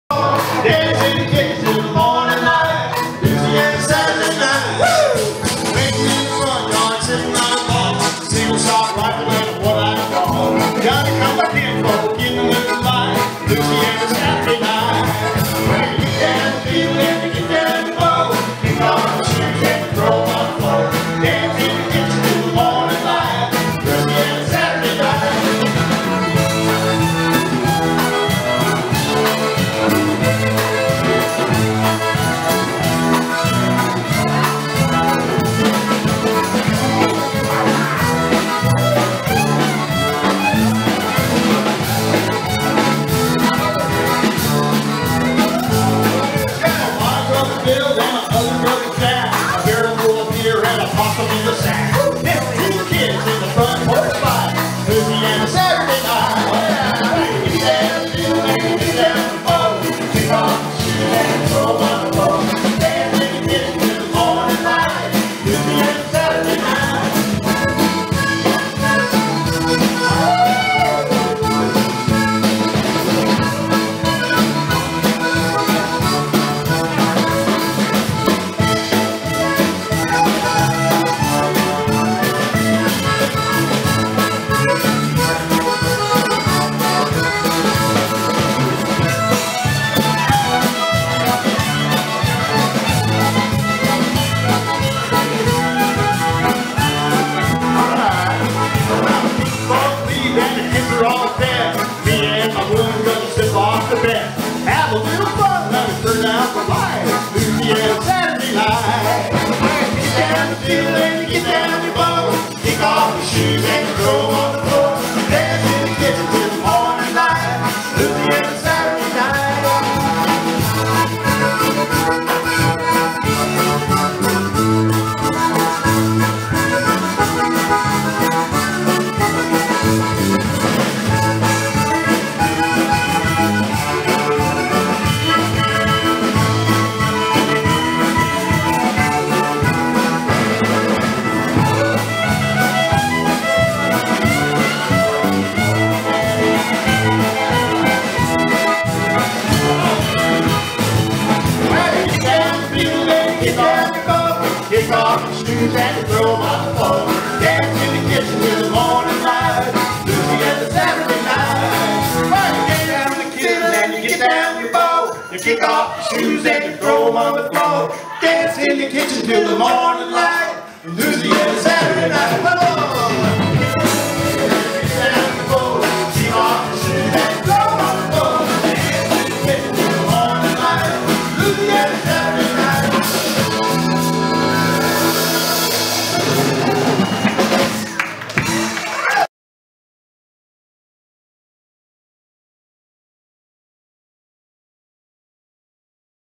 Smitty West Zydeco Orchestra
From The Ojai California Mardi Gras Wake-up Krewe's 2014 Sunday Fais Do Do!